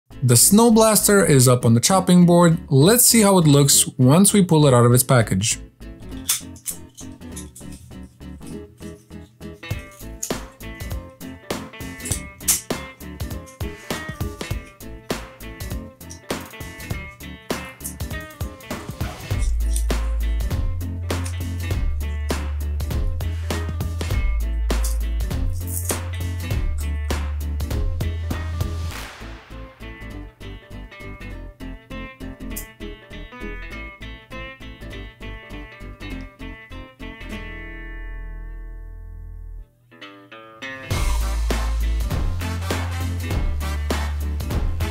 What's Inside the Snow Blaster Toy Mini Brands Series 2
Find out what's inside the Toy Mini Brands Series 2 Snow Blaster!